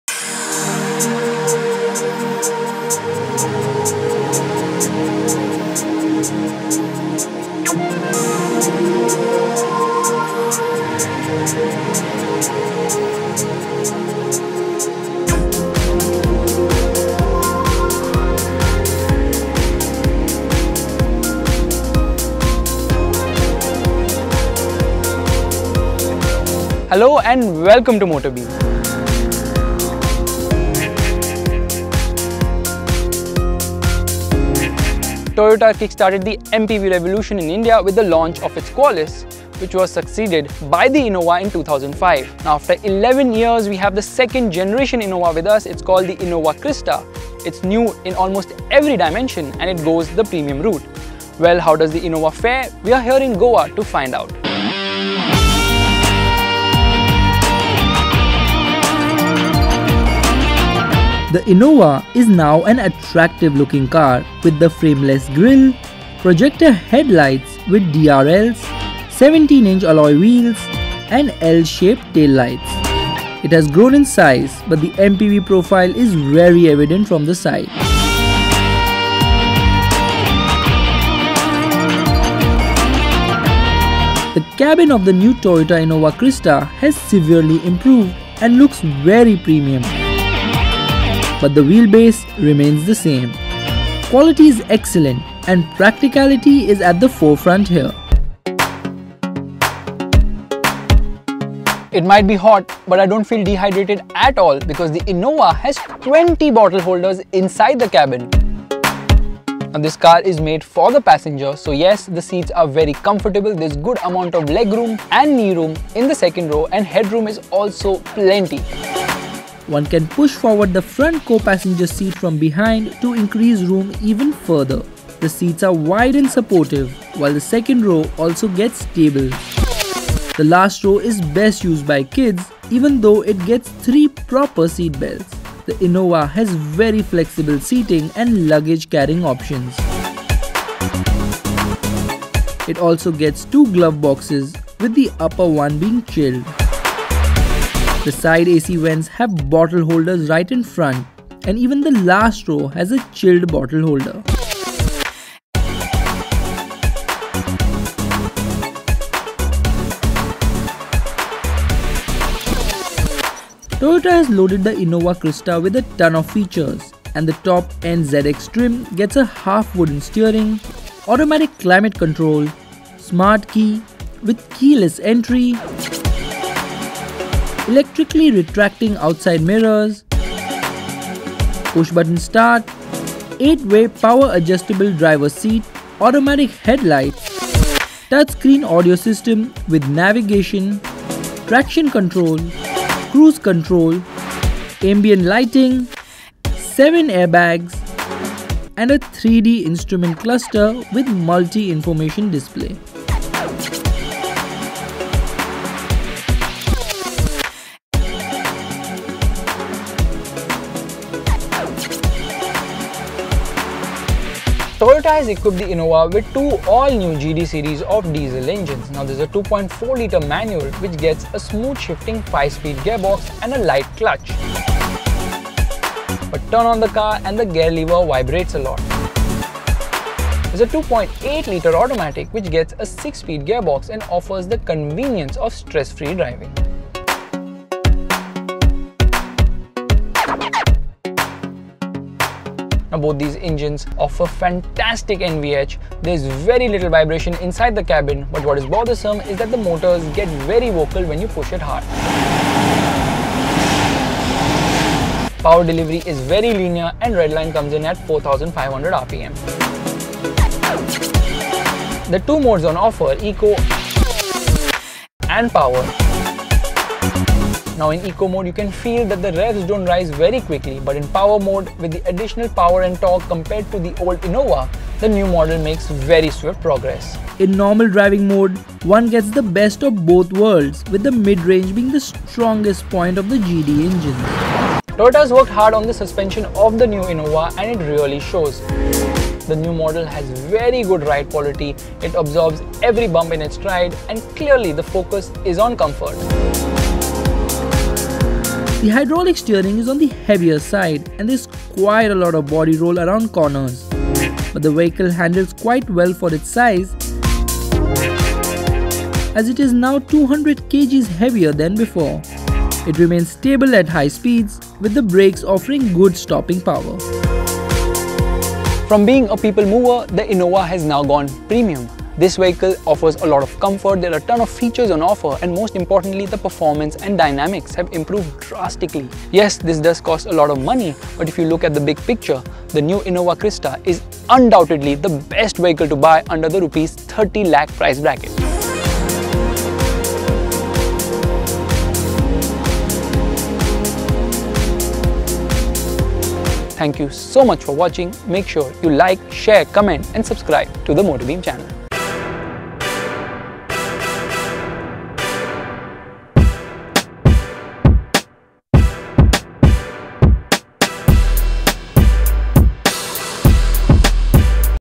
Toyota Innova Crysta Review - Best MPV In India | MotorBeam
tests the all-new, second generation Toyota Innova Crysta which has been long awaited. The first gen Innova was launched way back in 2005 and Toyota took 11 years to bring its replacement. The 2017 Toyota Innova Crysta is offered with a slew of equipment, it also gets the option of two diesel engines with an automatic gearbox also being offered.

The 2016 Toyota Innova Crysta is aimed at those looking for a comfortable MPV which is high on features and practicality. Read the complete text review of the Toyota Innova Crysta